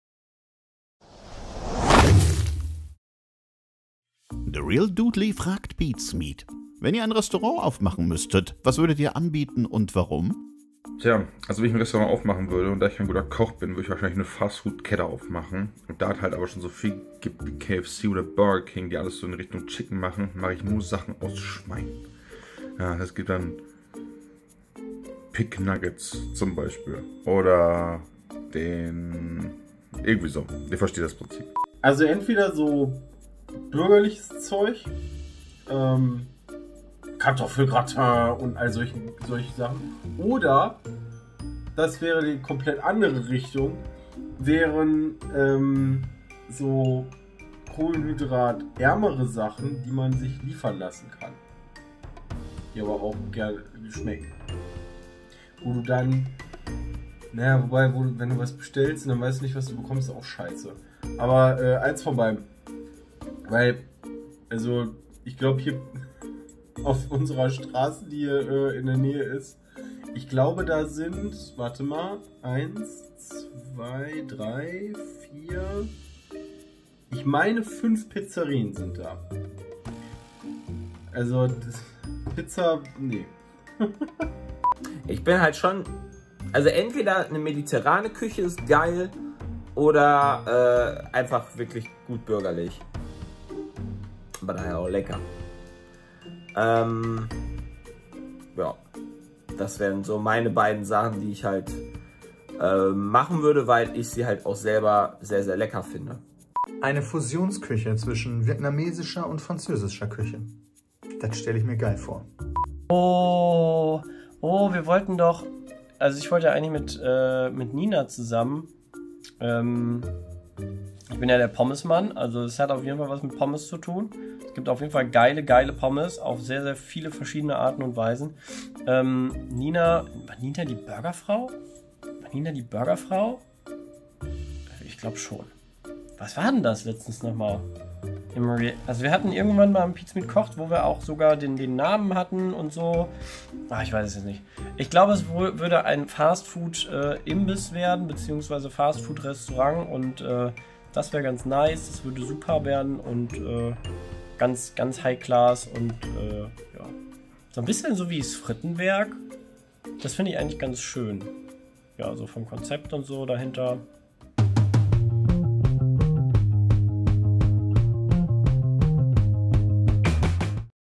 Was würdet ihr in eurem Restaurant anbieten? | Frag PietSmiet #2359
Was wären wohl die Spezialitäten?